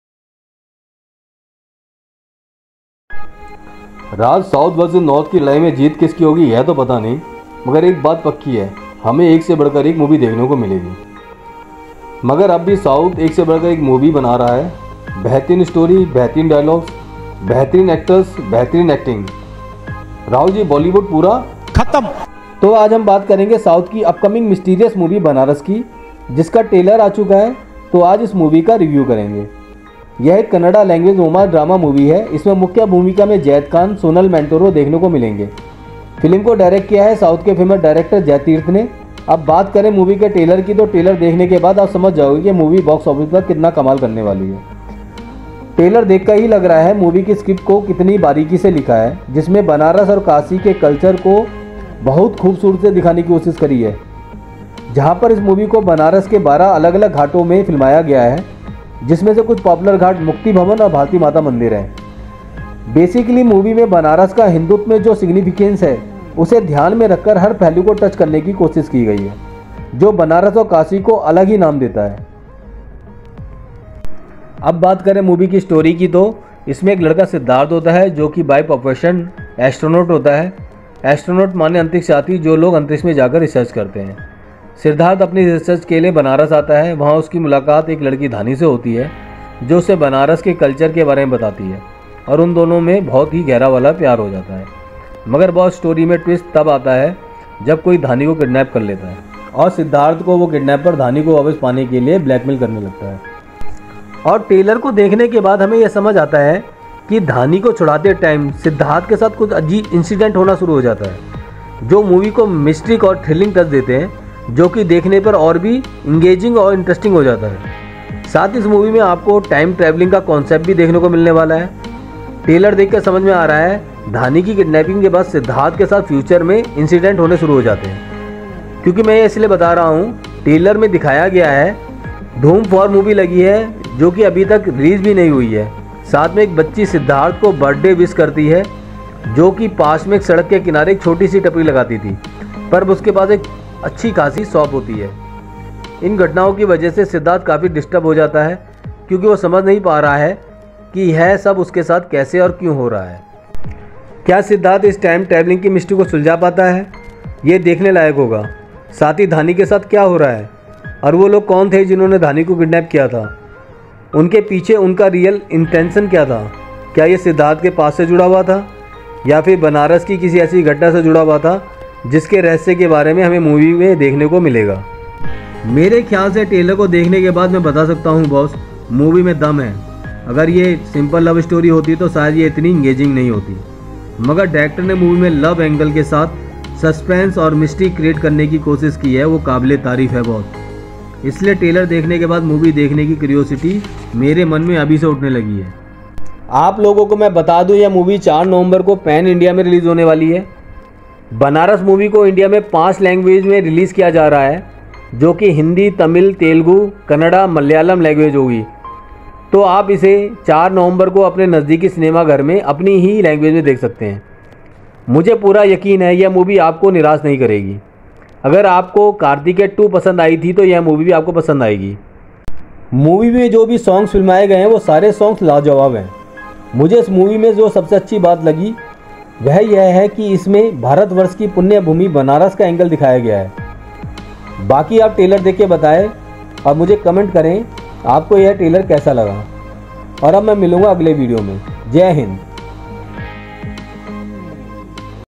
Banaras (2022) Upcoming Movie Trailer Review !! by Rajesh Pandey
Hey this is Rajesh Pandey, In this video i am review upcoming banaras movie trailer.

banaras movie (2022)